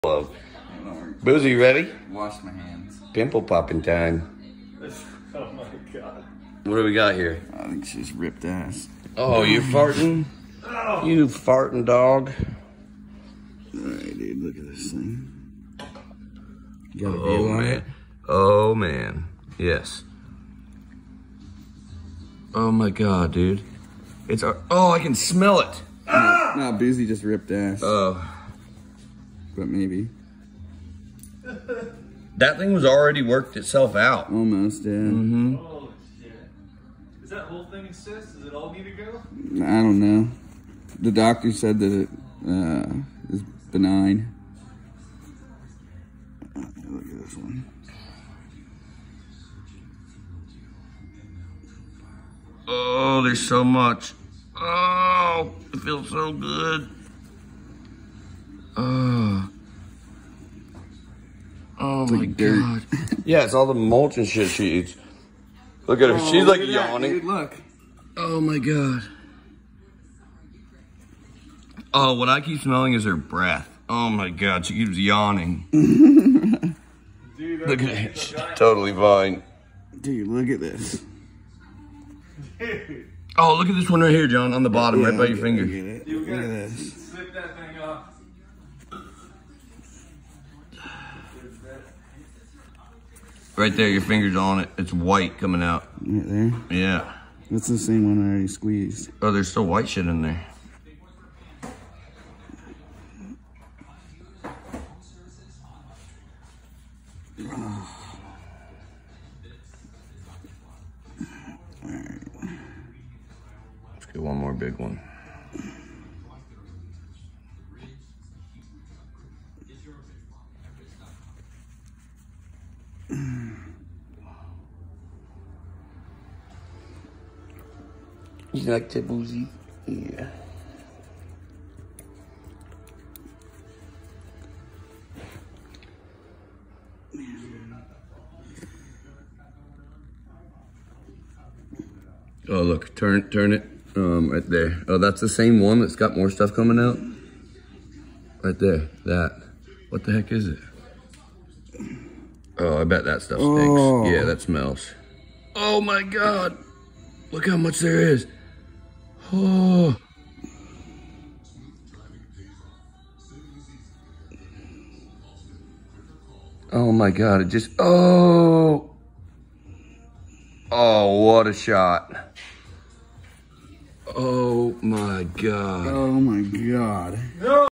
GIANT BLACK HEAD EXPLOSION on Pit Bull/WATCH TILL THE END!!!
My dog is old and has a tumor that’s benign and is full of black heads. Her vet said it’s not worth removing and squeezing it with washed hands is fine.